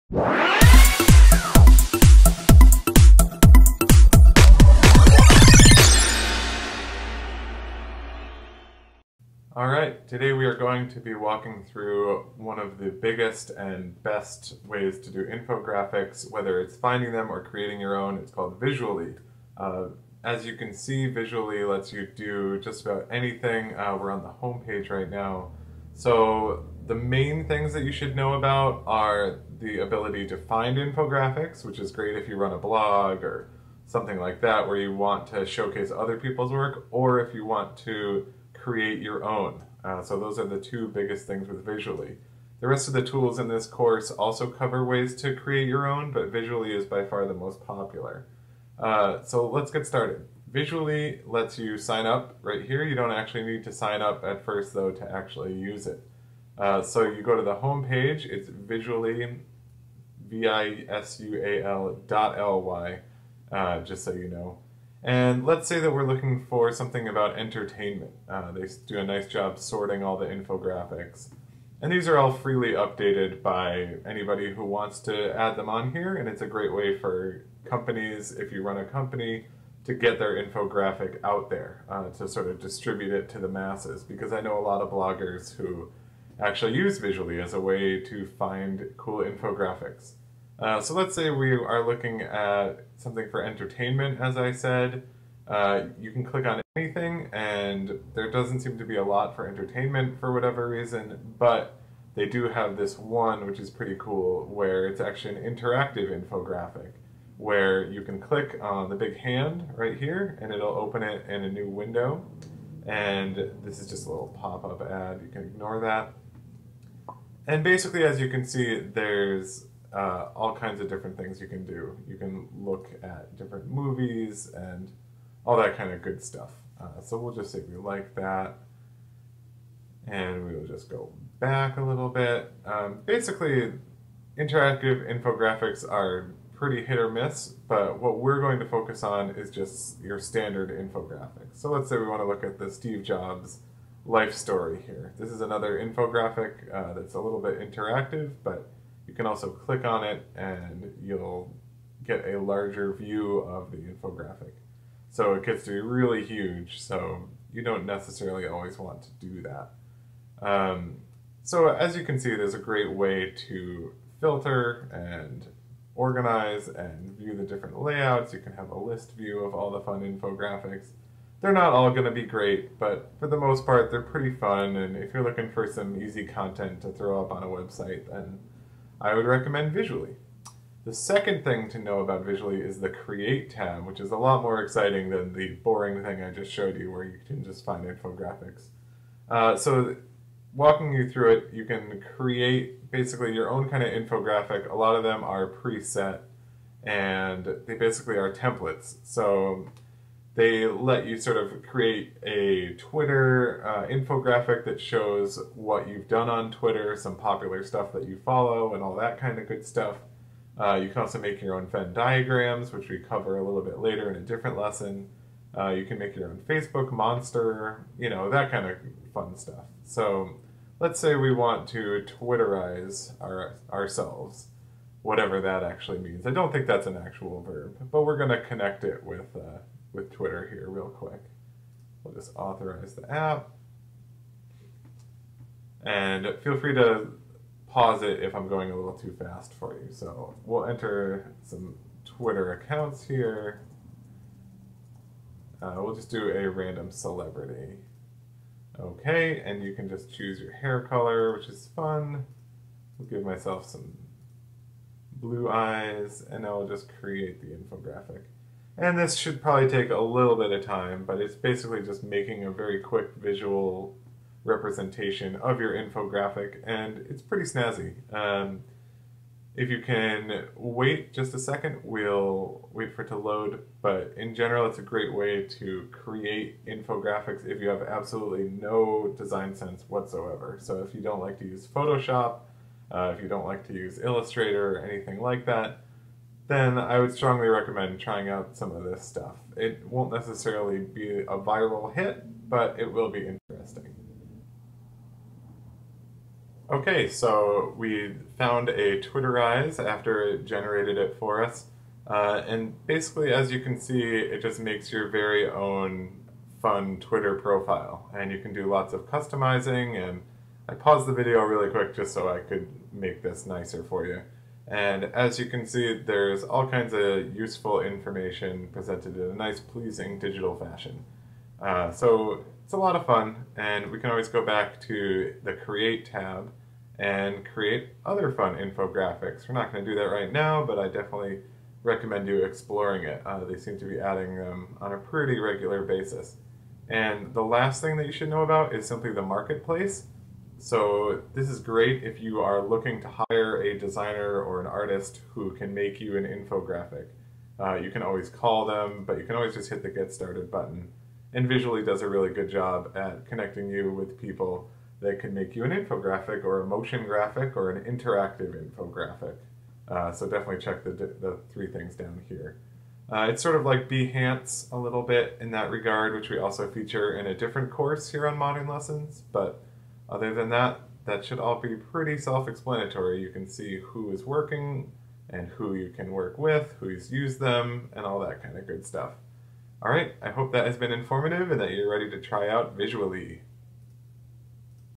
How To Get Started With Visual.ly
A quick walkthrough on the basics of the popular site Visual.ly!